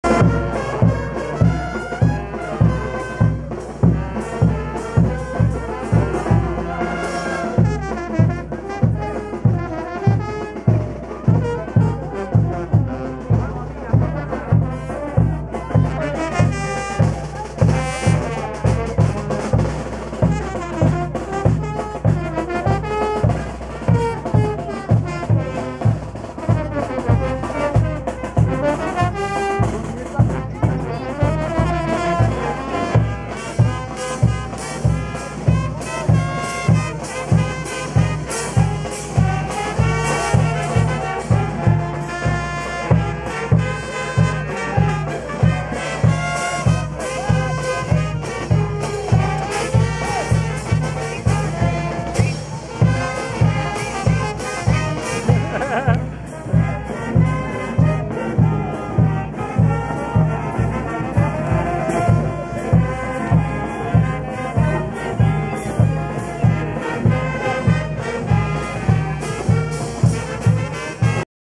Peruvian Style Wedding Celebration
One of the activities we were doing for the Lloqe Hatun Karpay was at a church in the hills close to Lake Titicaca. It so happened that the very Sunday we had the this activity there was a wedding celebration there. And the people of this area certainly know how to celebrate "in style" as we found out.